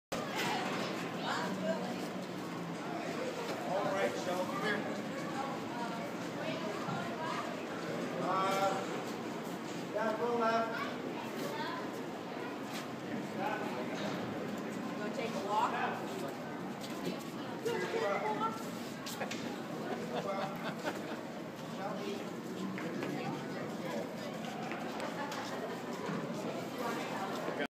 Elephant/Bull Hook
2014 Shrine Circus Fort Wayne, IN. An elephant follows a handler after he grabs a bull hook for control.